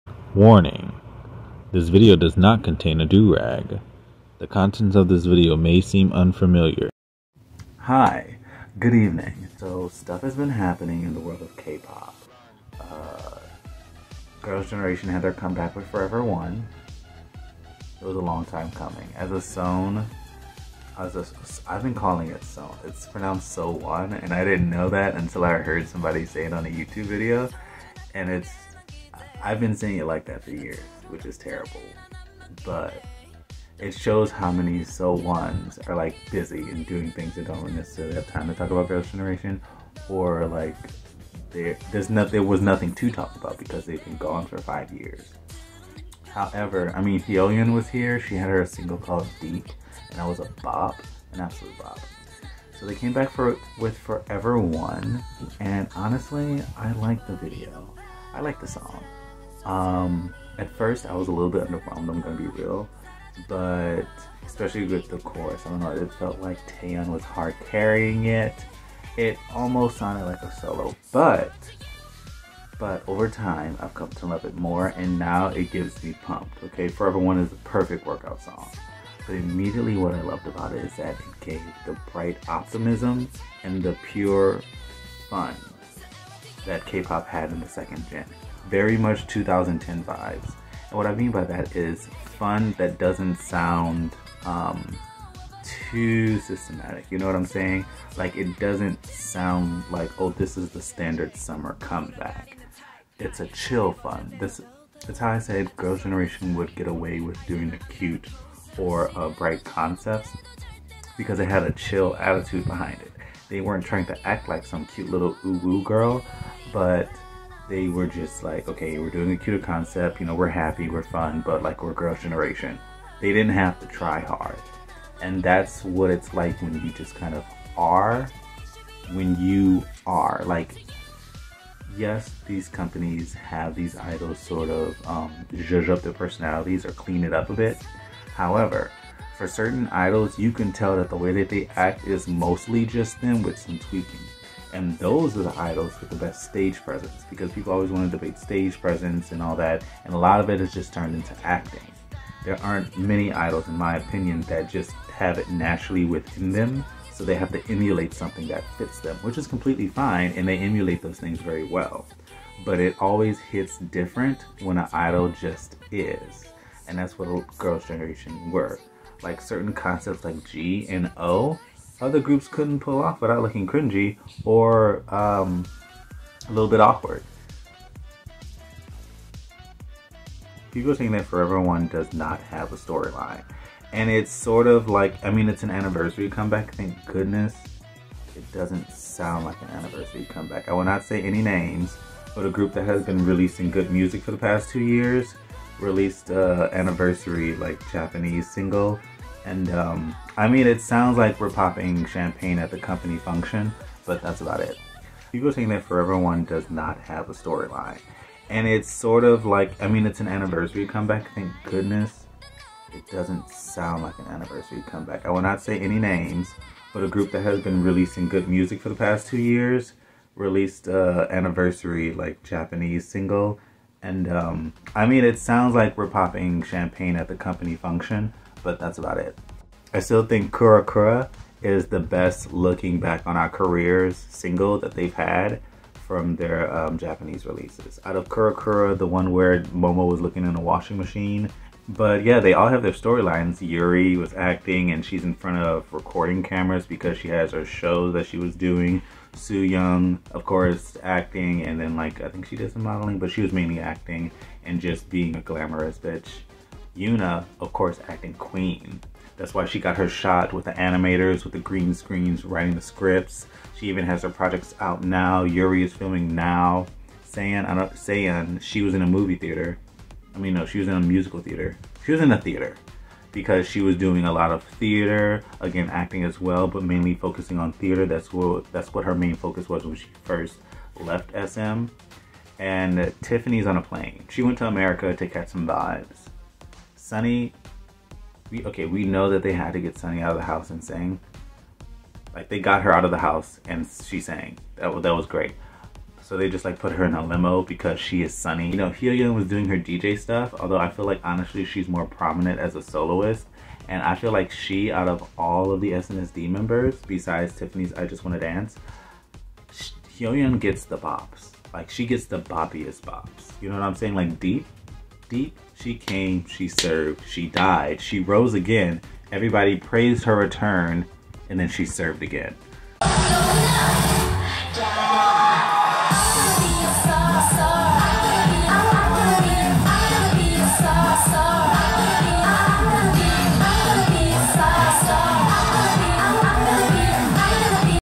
Let's Chat: Girls Generation vibes
quick. lil chat about snsd. next im reacting to twice